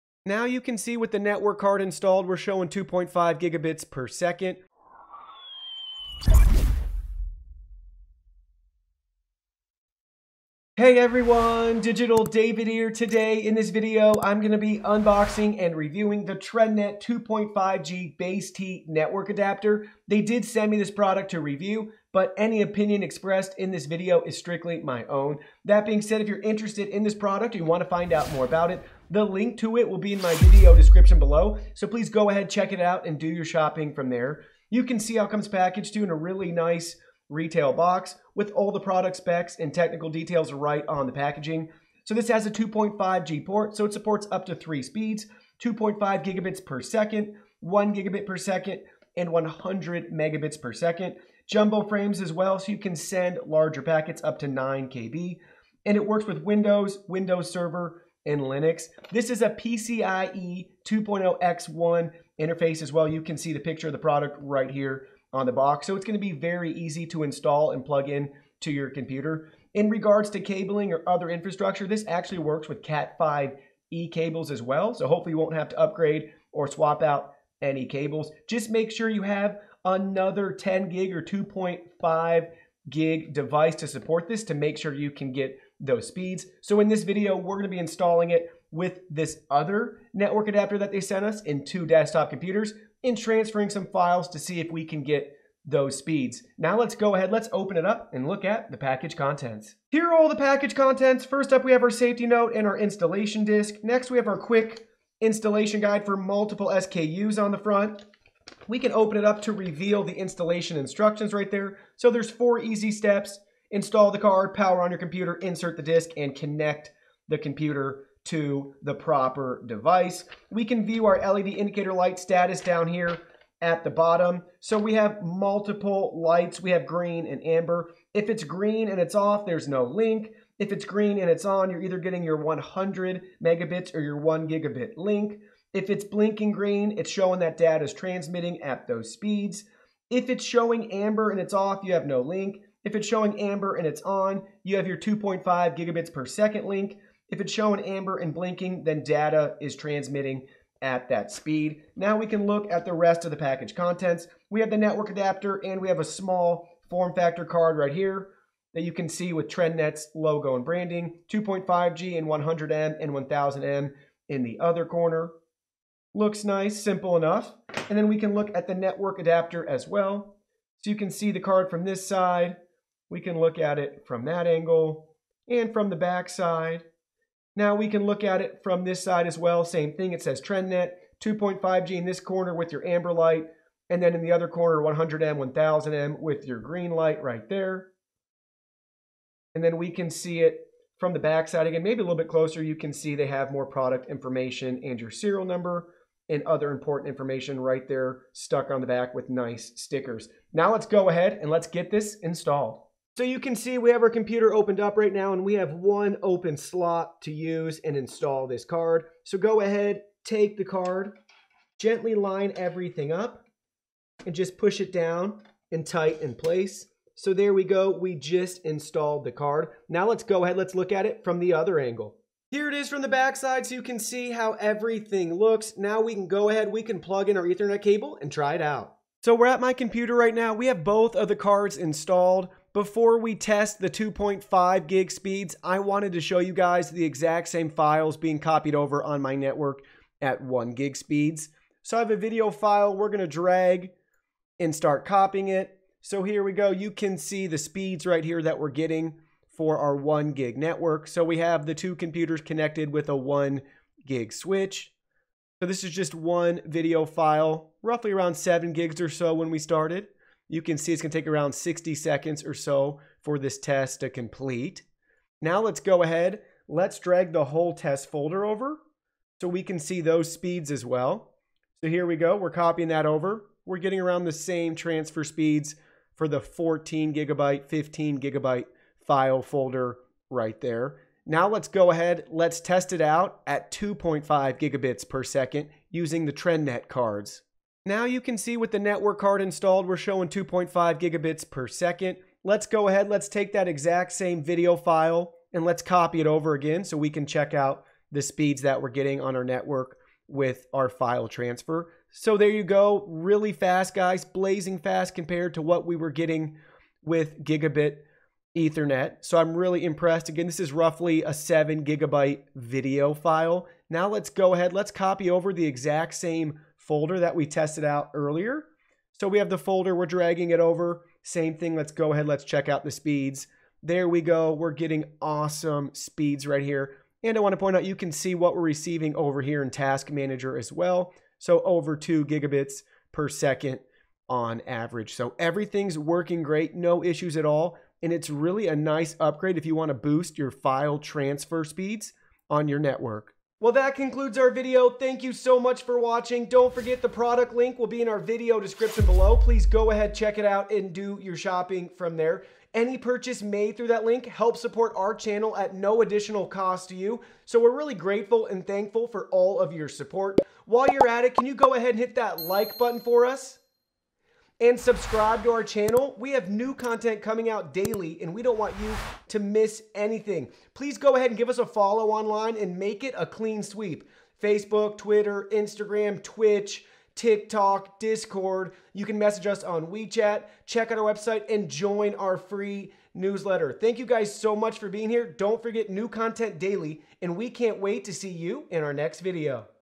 TRENDnet 2.5GBASE-T PCIe Network Adapter Installation // TEG-25GECTX Review
TRENDnet 2.5GBASE-T PCIe Network Adapter

TEG-25GECTX   (Version v1.0R)
■ Converts a PCIe slot into a 2.5G Ethernet Port
■ Supports 802.1Q VLAN Tagging
■ Includes standard and low-profile brackets
■ Windows® and Linux® support
■ IEEE 802.3ab and 802.3bz compliant
■ Supports up to 9KB jumbo frames

TRENDnet TEG-25GECTX, 2.5G Base-T PCIe Network Adapter, Standard and Low-Profile Brackets Included, Windows, Server, Linux and Vmware Esxi 6.X, 5.X